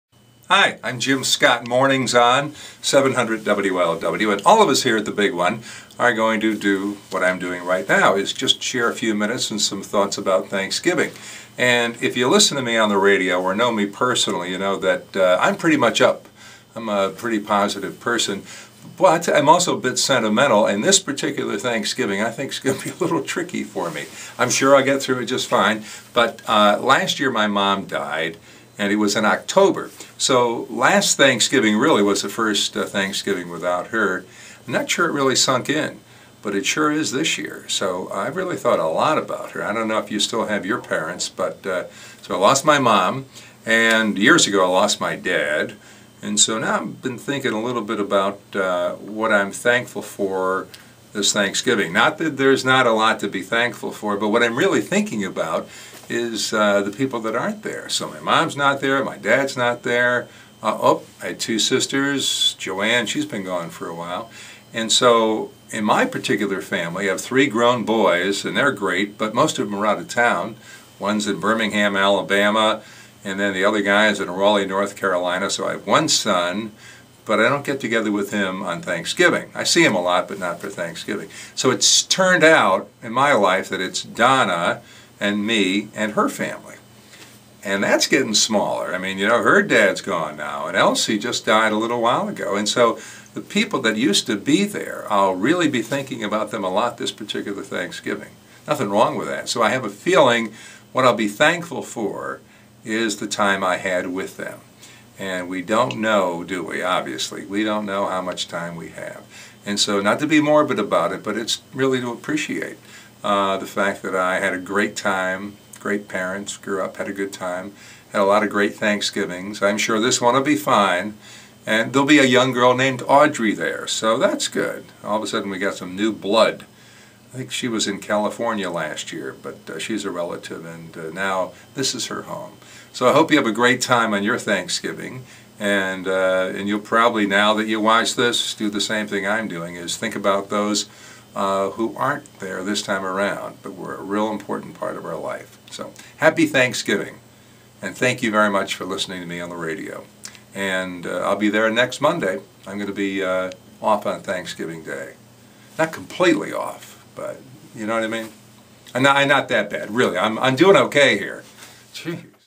Jim Scott shares what he is thankful for
This Thanksgiving the Big One is sharing our Thanksgiving stories and what we're thankful for with you.